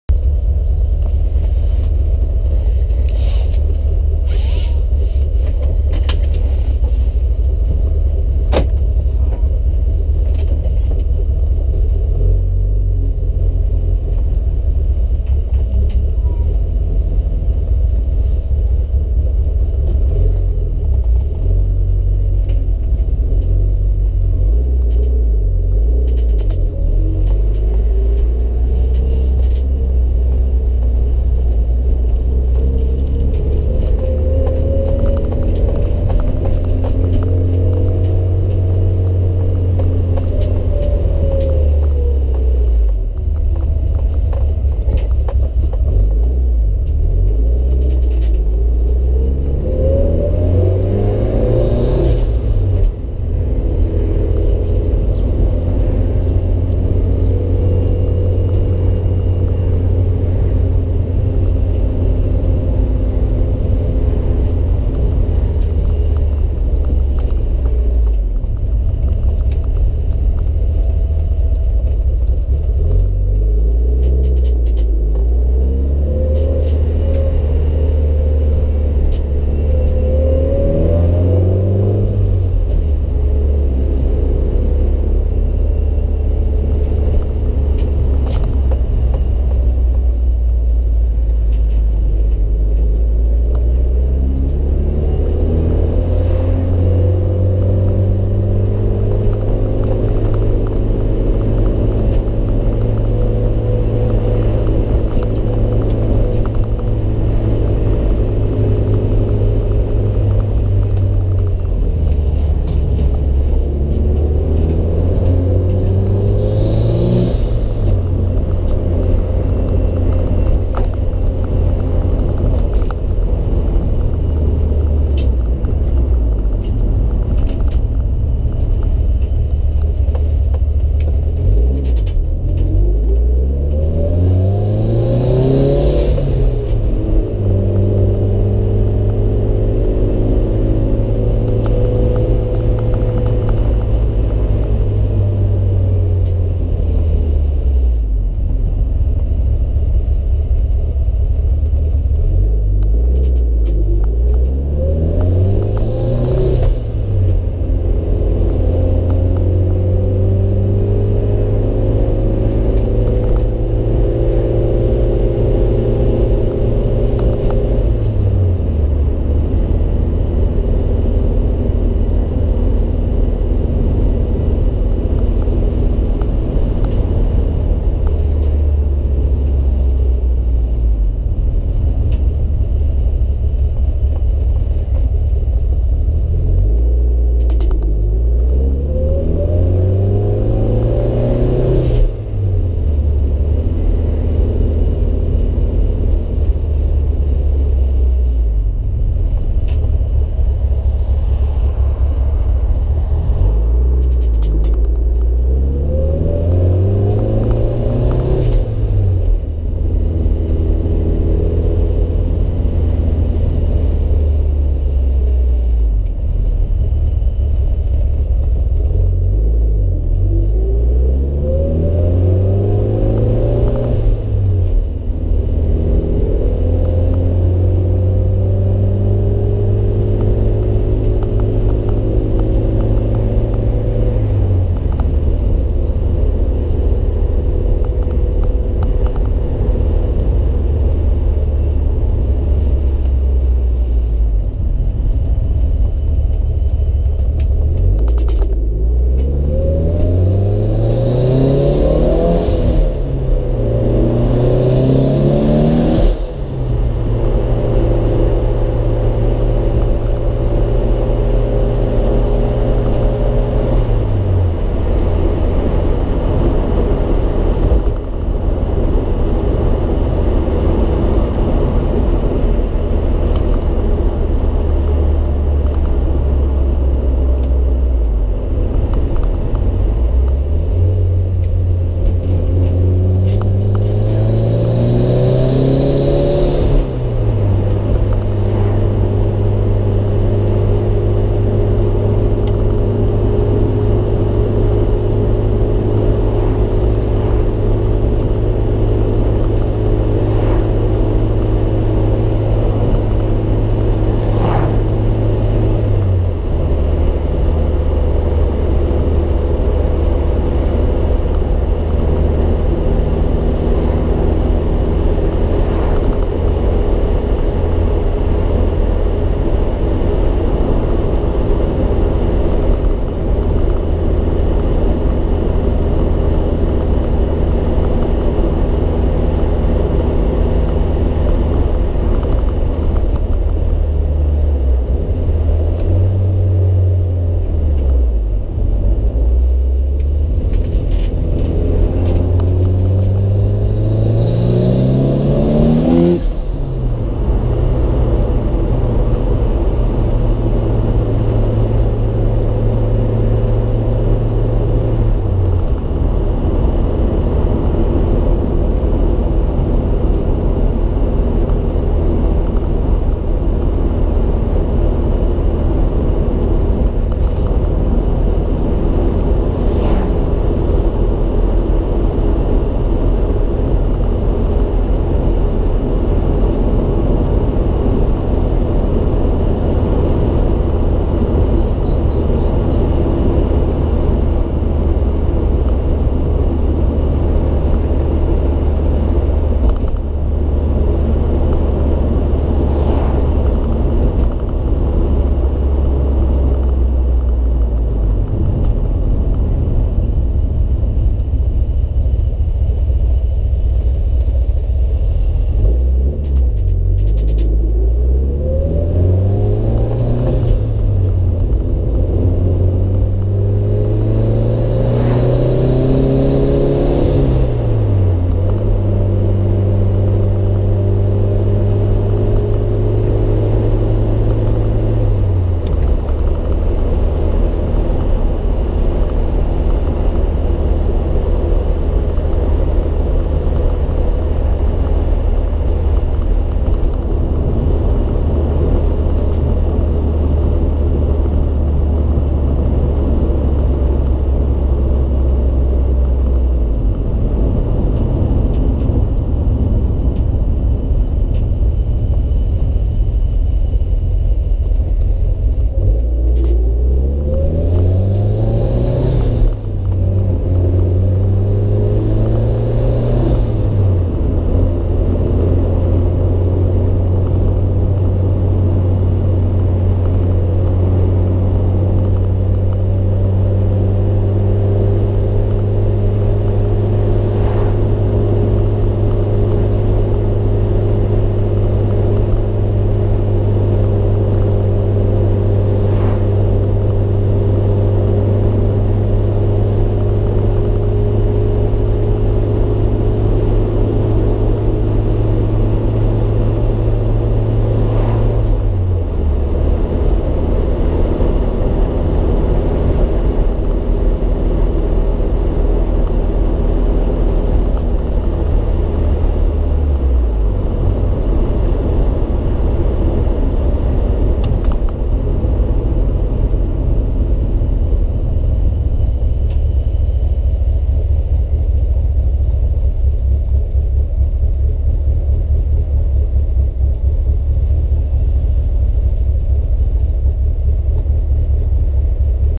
Evo 8 Go Pro/Clutch Break In
Just breaking in the new Stage 3 Competition Clutch taking some back roads. No boost or pulls sorry guys!